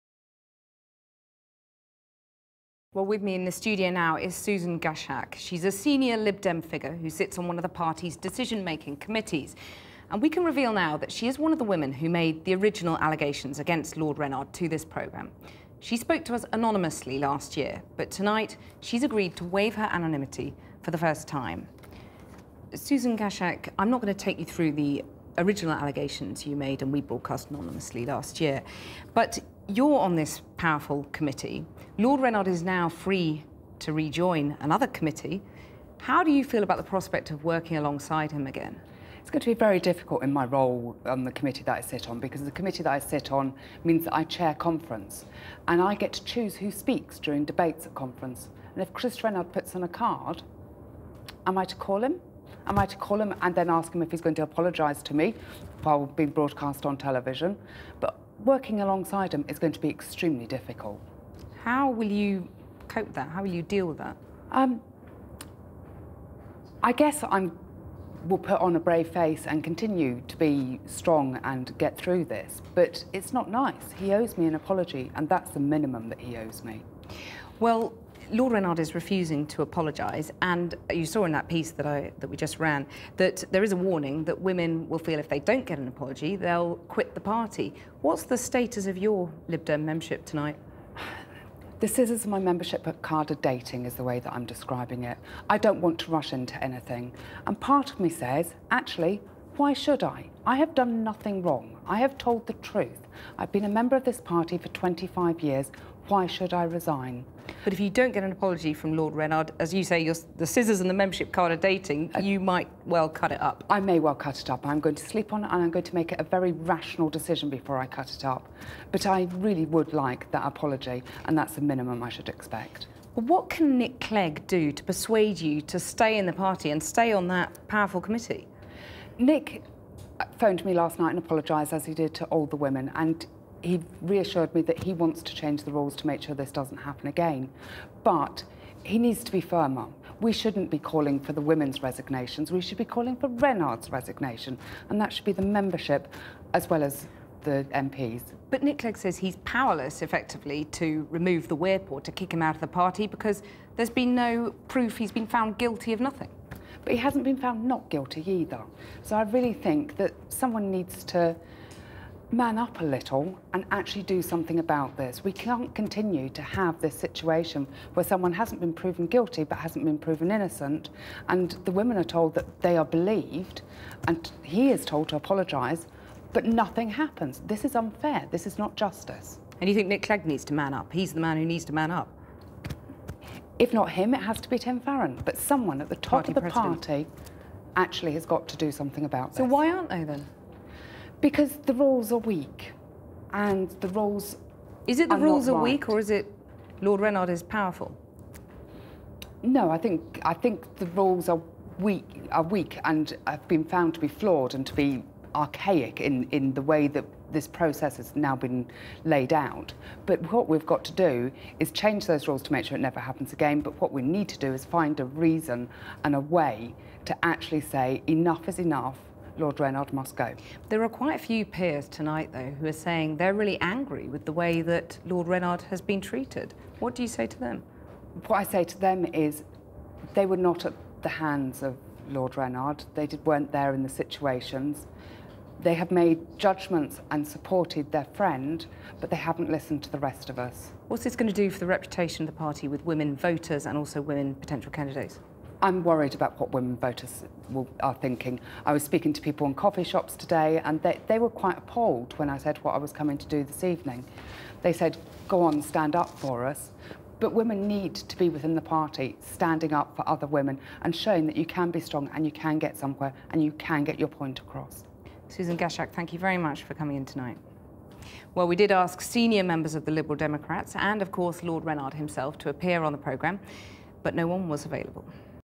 'Enough is enough, Lord Rennard must go' - video
Cathy Newman speaks to Susan Gaszczak, a senior Lib Dem figure, who was one of the women who made the original allegations against Lord Rennard to this programme.